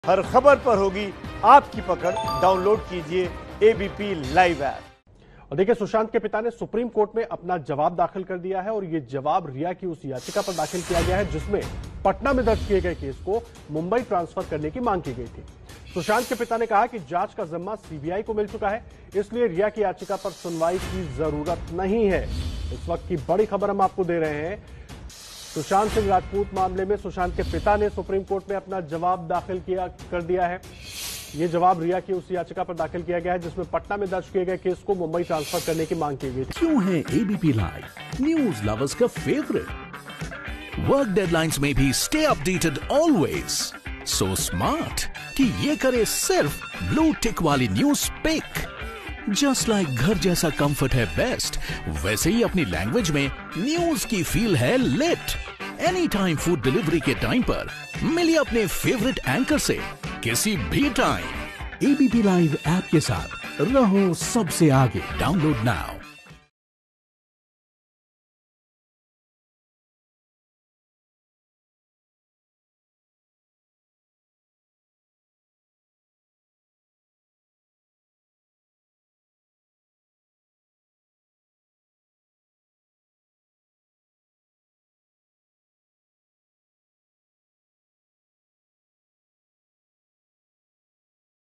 Sushant Singh Rajput के पिता ने Supreme Court में दायर किया अपना पक्ष | ABP News Hindi
सुशांत सिंह राजपूत के पिता ने सुप्रीम कोर्ट में कहा है कि रिया चक्रवर्ती की पटना में दायर FIR को मुंबई ट्रांसफर करने की याचिका पर सुनवाई नहीं होनी चाहिए क्योंकि मामला अब CBI के पास पहुंच चूका है. ऐसे में इस याचिका का अब कोई महत्त्व नहीं रह जाता है.